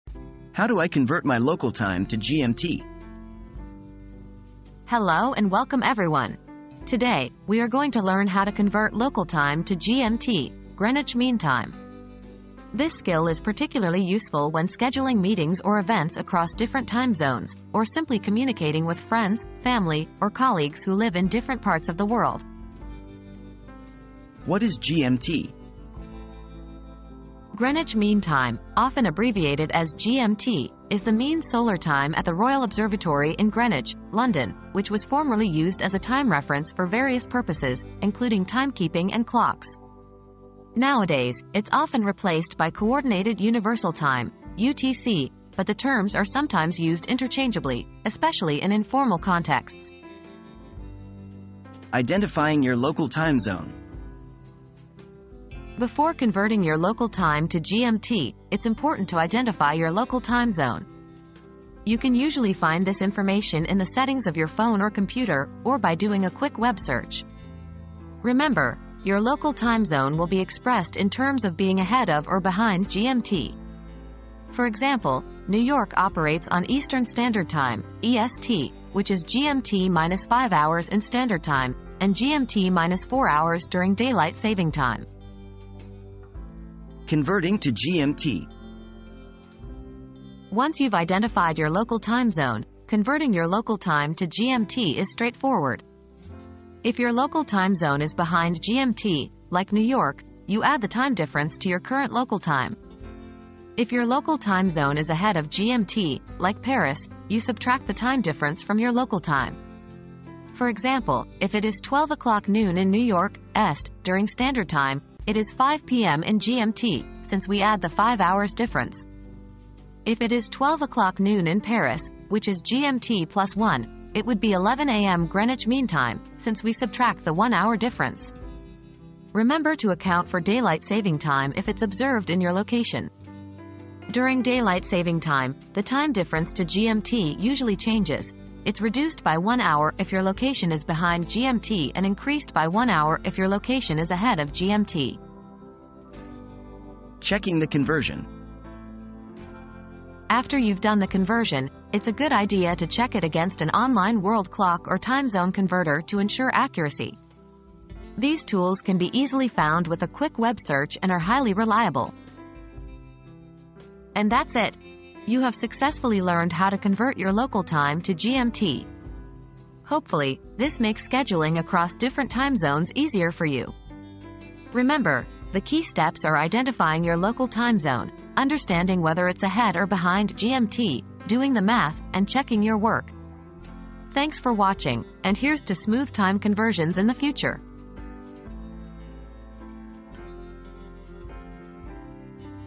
How do I convert my local time to GMT?
Converting Local Time to GMT: An Easy Guide • Learn how to easily convert your local time to GMT (Greenwich Mean Time) in this informative video! Whether you're scheduling meetings across time zones or connecting with friends and family worldwide, this skill is essential. Discover what GMT is, identify your local time zone, and master the conversion process. Don't miss out on this handy guide!

00:00 • How do I convert my local time to GMT?
00:24 • What is GMT?
00:51 • Identifying Your Local Time Zone
01:27 • Converting to GMT
02:27 • Checking the Conversion